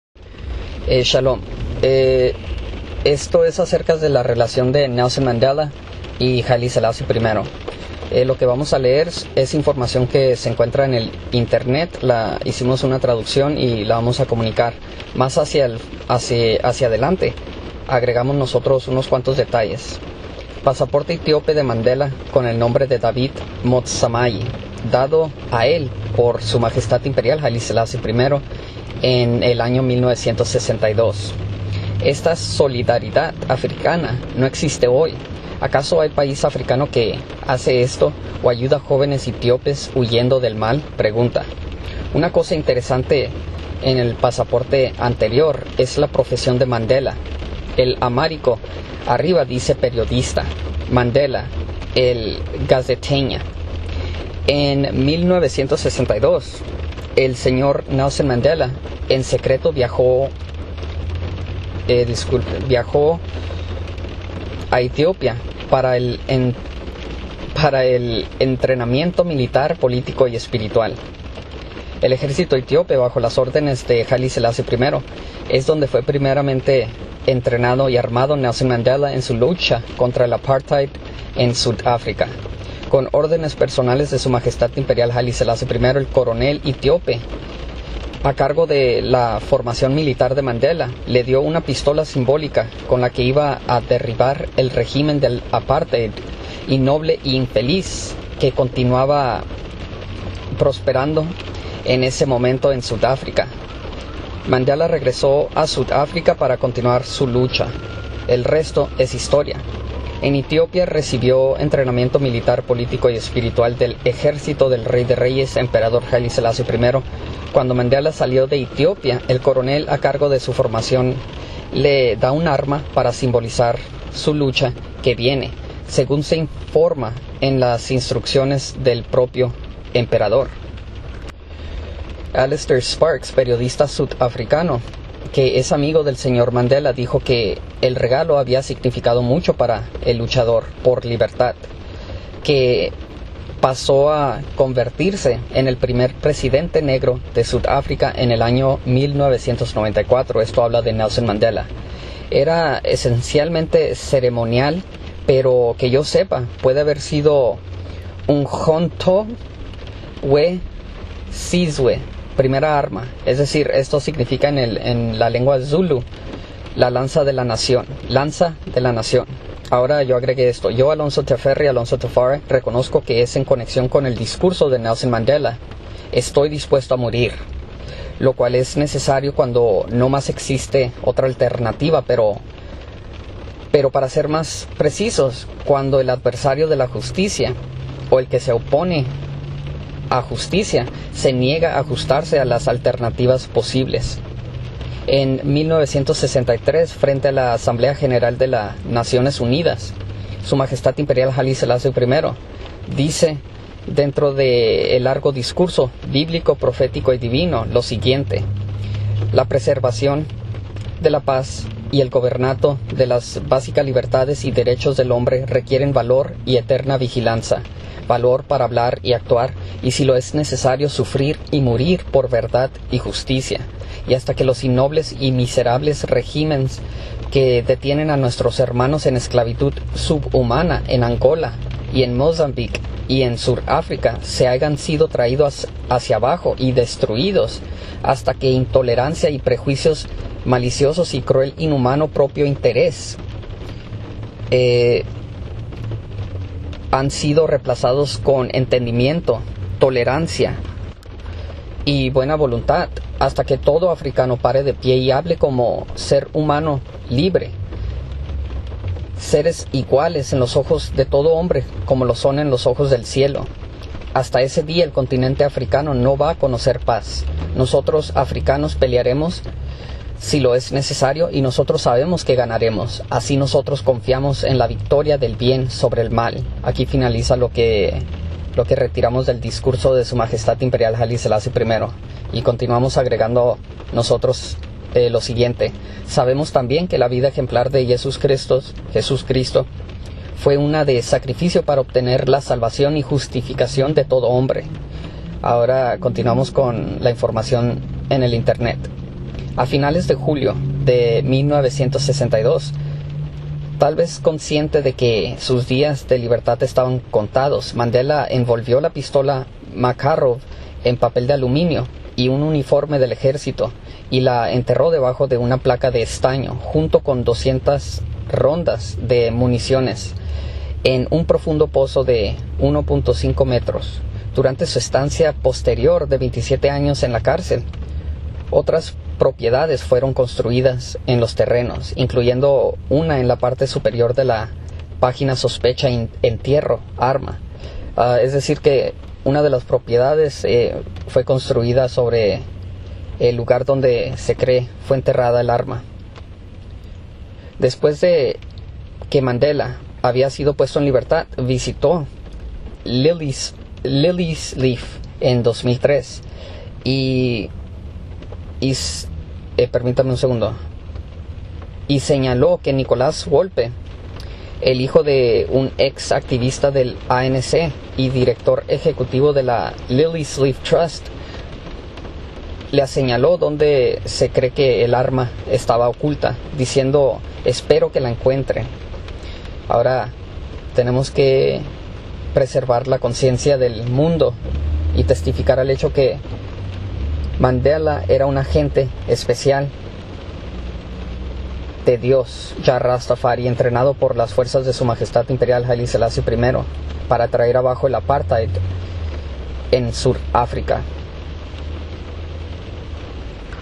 HAILE SELASSIE I Destruyo APARTHEID en SUR-AFRICA-NELSON MANDELA ENTRENADO EN ETHIOPIA
LOJSocietyEspañol-"SOCIEDAD LEON DE JUDA"_ "La preservacion de paz y el gobernato de las basica libertades y derechos del hombre requieren valor y eterna vigilanza. Valor para hablar y actuar y si es necesario sufrir y murir por verdad y justicia." "Y hasta que los inobles y meiserables regimens que detienen a Nuestros Hermanos en esclavitud sub-humana en Angola y Mosambique y en Sur Africa se haigan sido traidos abajo y destruidos. Hasta que intolerancia y prejuicios, malisiosos y cruel inhumano propio interes han sido replasados con entendimiento, tolerancia y buena voluntad, hasta que todo Africano para de pie y hable como ser humano libre. Seres iguales en los ojos de todo hombre como lo son en los ojos del cielo. Hasta ese dia el continente Africano no va conocer paz. Nosotros Africanos peliaremos si lo es necesario y Nosotros sabemos que ganaremos, asi Nosotros comfiamos en la victoria del bien sobre el mal." HAILE SELASSIE I